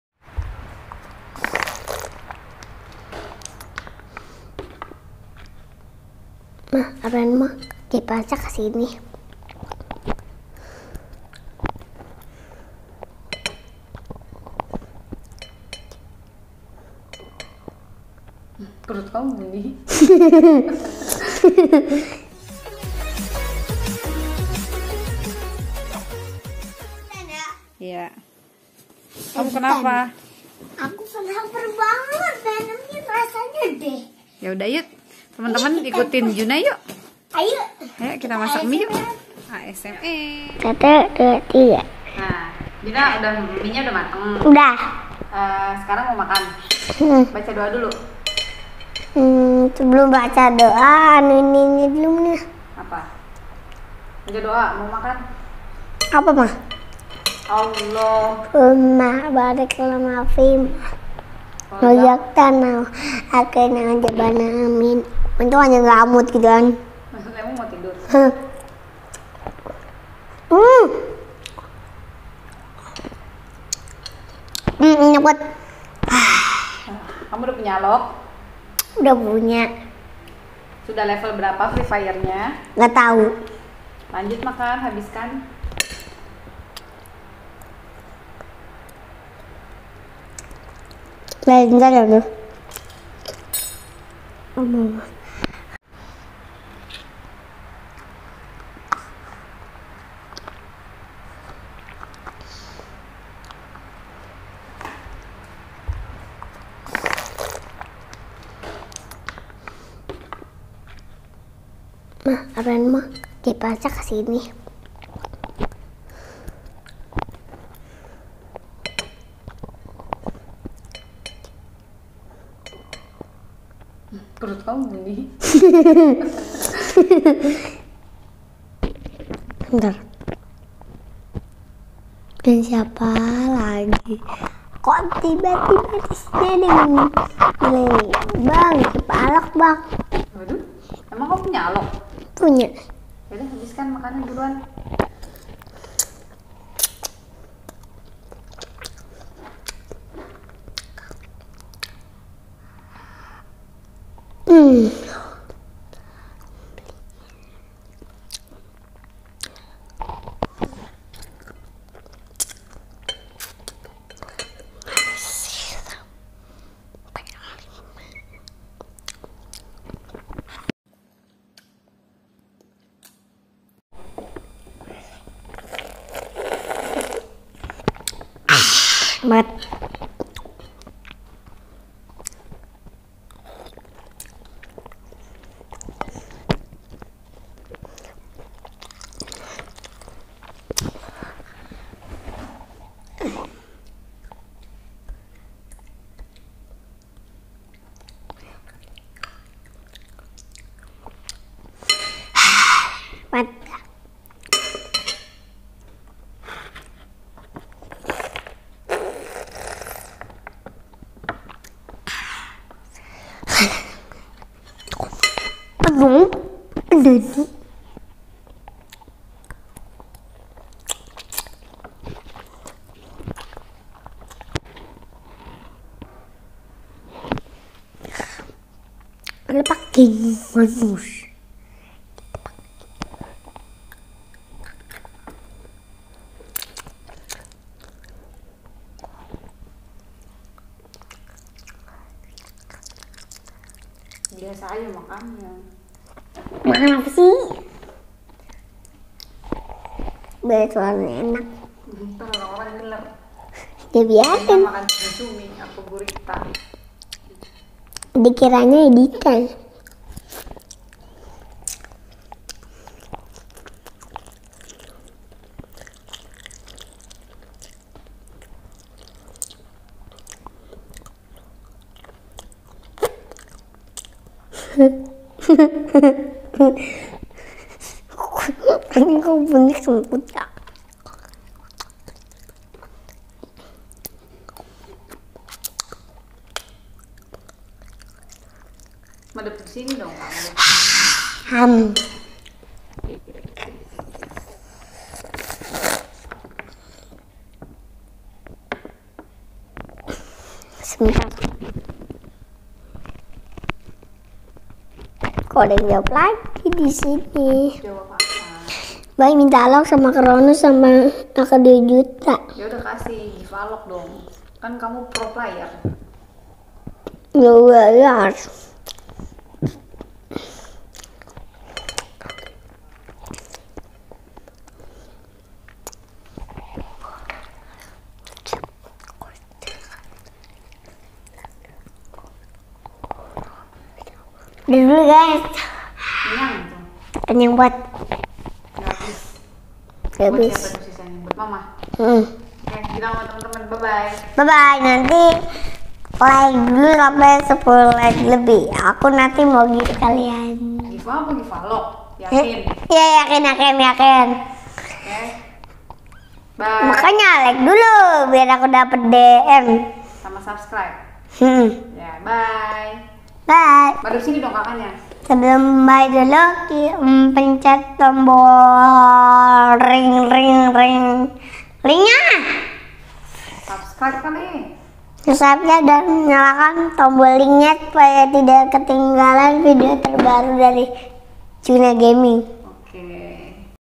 Asmr indomie goreng | mukbang indomie
Bismillahirrahmanirrahim
Kali juna kelaperan guys jadi dia minta makan indomie goreng
Kita coba coba pake microphone 12 ribuan neh.